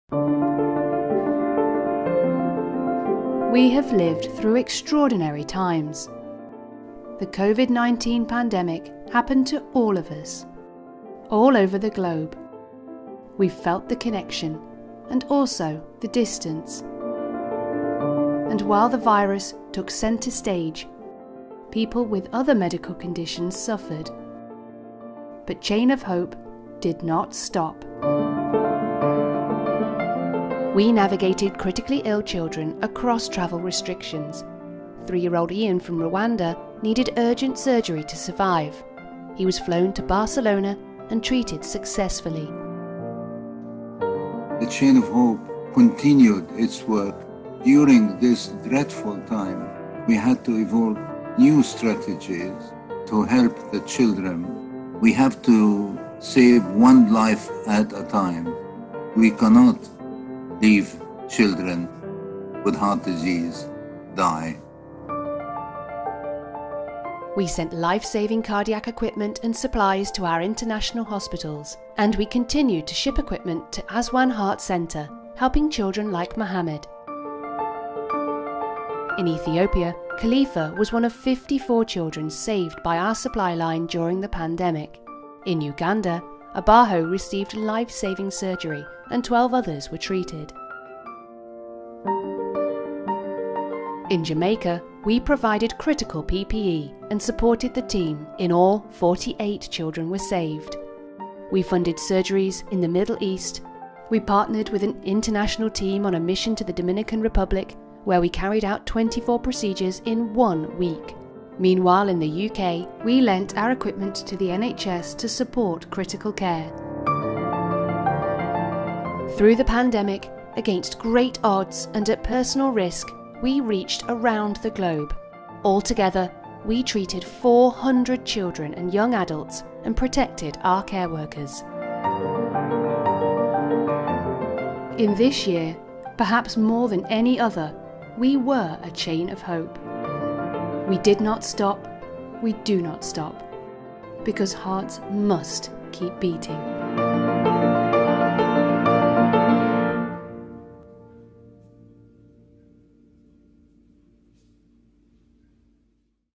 Chain of Hope - Saving Lives During the Pandemic.
Despite the ongoing effects of the Pandemic, Chain of Hope did not stop, we successfully treated over 400 children since April 2020 when the pandemic hit.